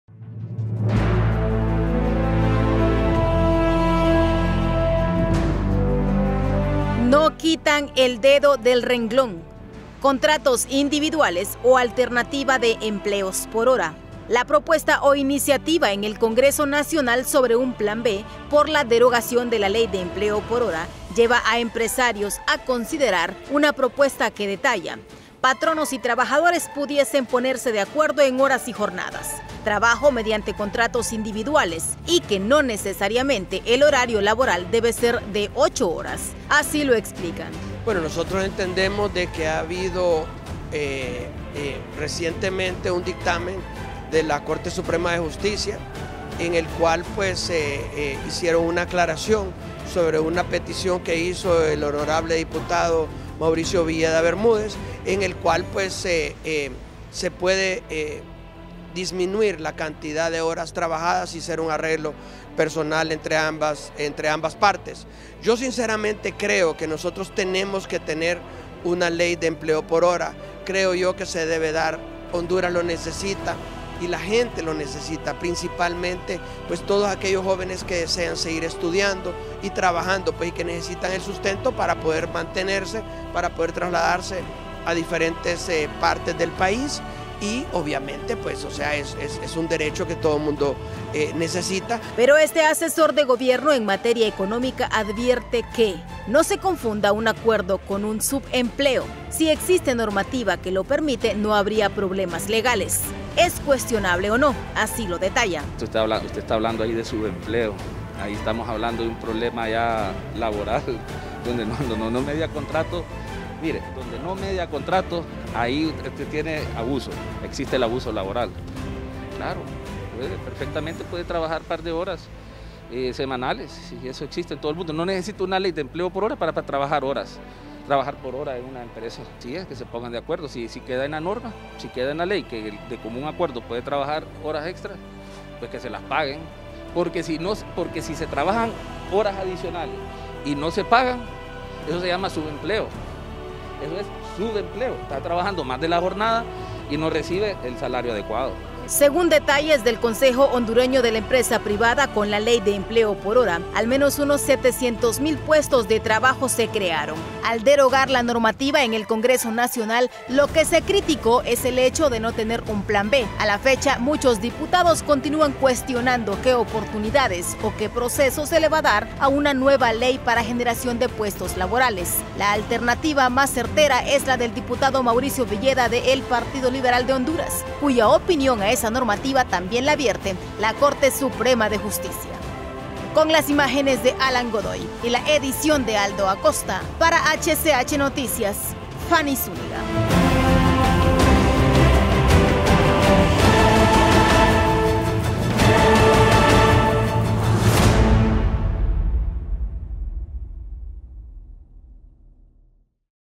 Vuelve a la palestra el recién eliminado Empleo por Hora en Honduras
Un “Plan B” para contratos individuales o alternativa de empleos por hora, es la propuesta o iniciativa de los empresarios.